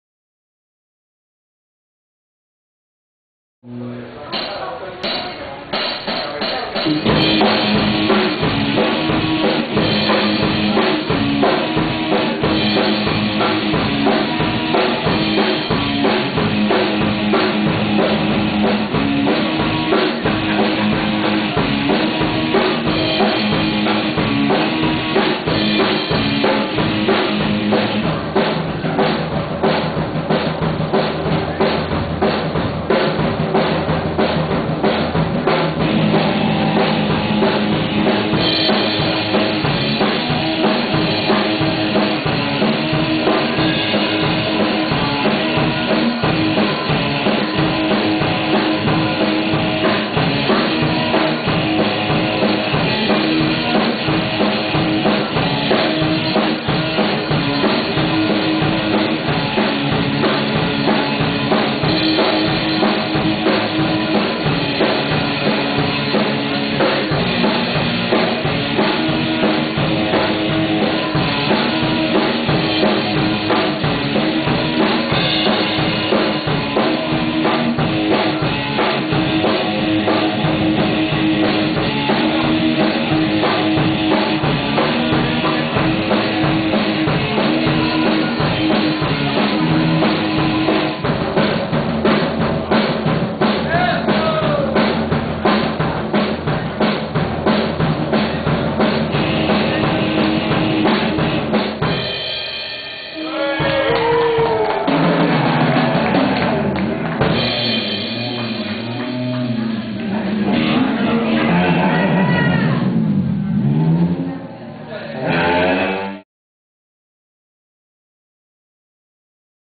The Music Store - Puerto Rico
Naim Arsuaga playing a Tama Bubinga drum set & Ignacio Fernandez playing a DBR Bolero Guitar